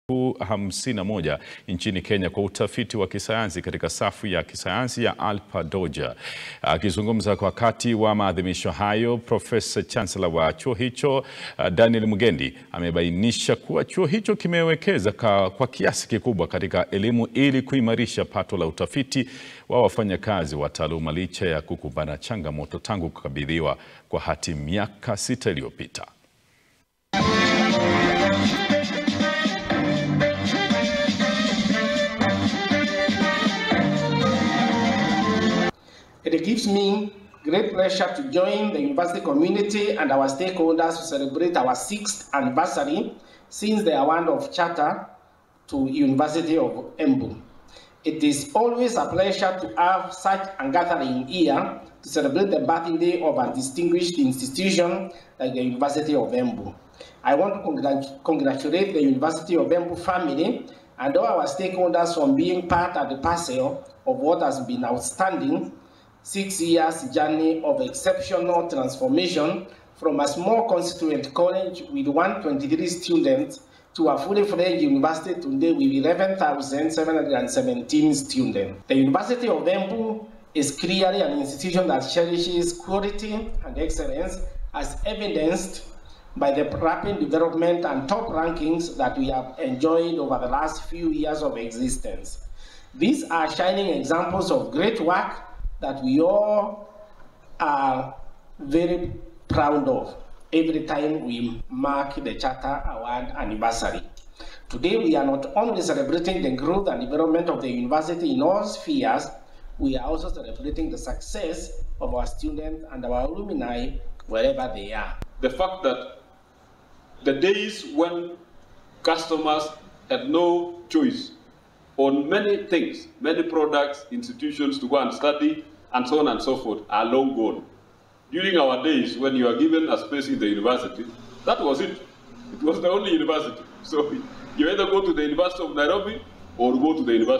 Chuo kikuu na elimu
Chuo kikuu cha Embu chaadhimisha miaka sita baada ya kupokea hati.

Ni baadhi ya vyuo vinavyofanya vyema zaidi katika taasisi ya kisayansi nchini.